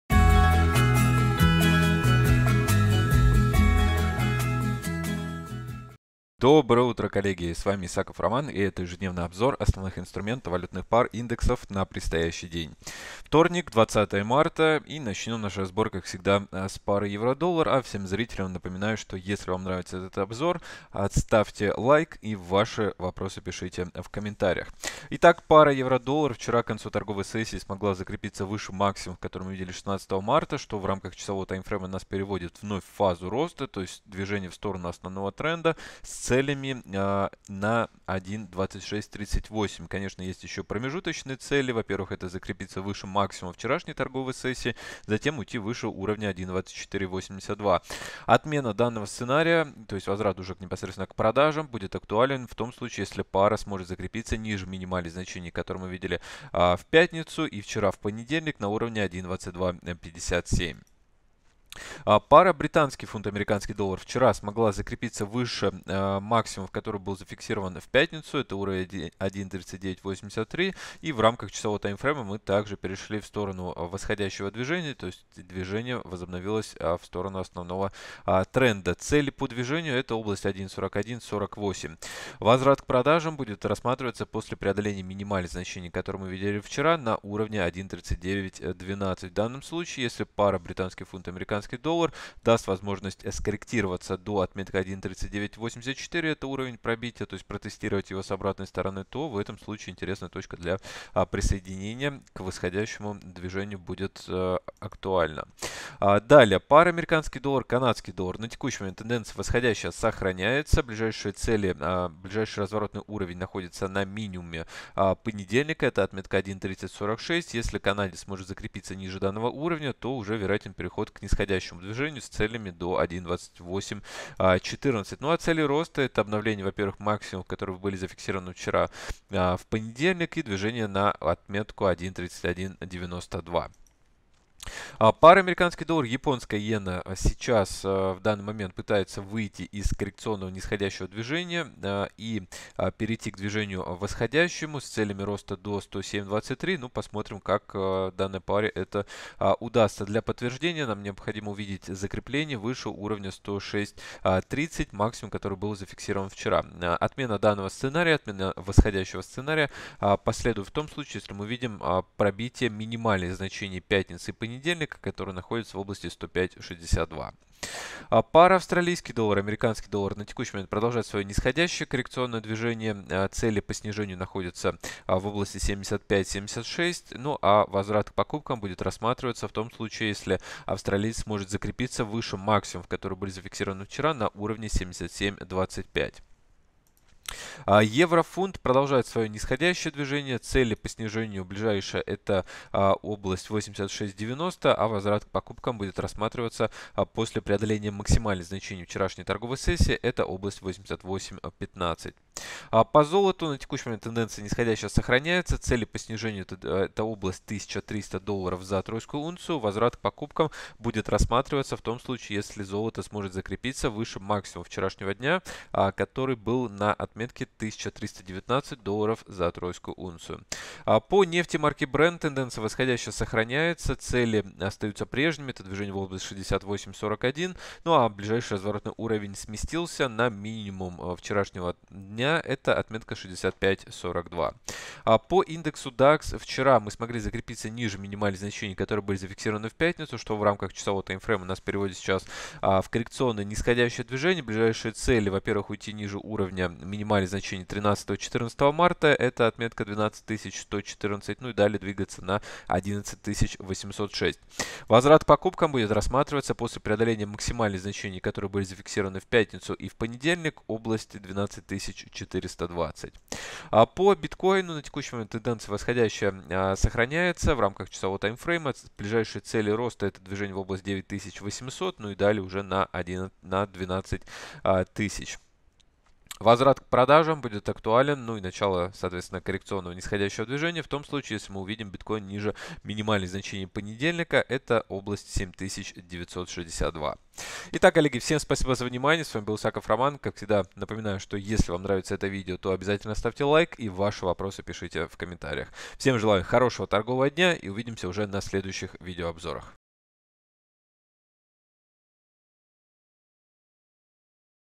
Аналитика рынка форекс на 20 марта: EURUSD, GBPUSD, GOLD, Brent, DAX30, Биткоин
Роман Исаков прогноз рынка форекс на 20 марта EURUSD, GBPUSD, AUDUSD, USDCAD, USDCHF, USDJPY, биткоин, золото, нефть, индекс DAX30
EURUSD   H1 - смена нисходящего направления на восходящее

GBPUSD H1 - смена нисходящего направления на восходящее

USDCAD H1 - сохраняется восходящая тенденция

USDJPY H1 - сохраняется нисходящая тенденция

EURGBP H1 - сохраняется нисходящая тенденция

GOLD H1 - сохраняется нисходящая тенденция

DAX30 H1 - смена восходящего направления на нисходящее

BTCUSD H1 - сохраняется восходящая тенденция